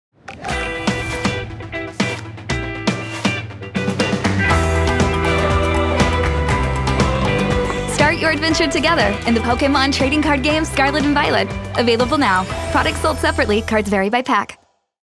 Journey to the Paldea region with the Pokémon TCG: Scarlet & Violet expansion
Set out for a journey in the Paldea region! Meet first partners Sprigatito, Fuecoco, and Quaxly, and explore the power of the Legendary Pokémon Koraidon and Miraidon as Pokémon ex. Other Pokémon ex evolve their way into battle—including Arcanine and Gyarados with a dazzling new look. Even more Pokémon appear as illustration rare cards with amazing artwork—discover them all in the Pokémon TCG: Scarlet & Violet expansion!